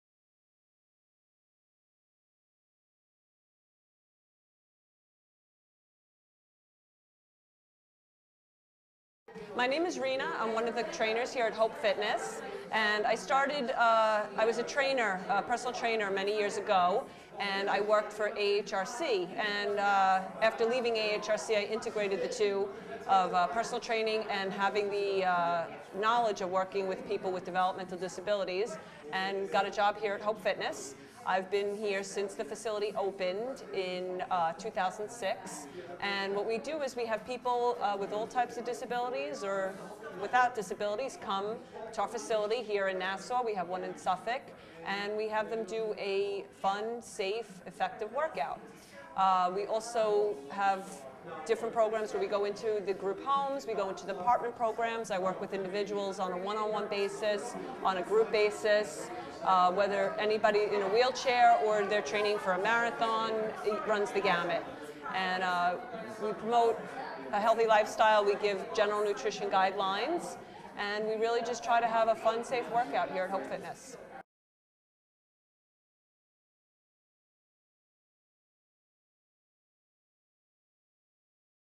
Rena's (Personal Trainer) Testimonial - HOPEFitness
Rena (Personal Trainer) speaks about HOPEFitness.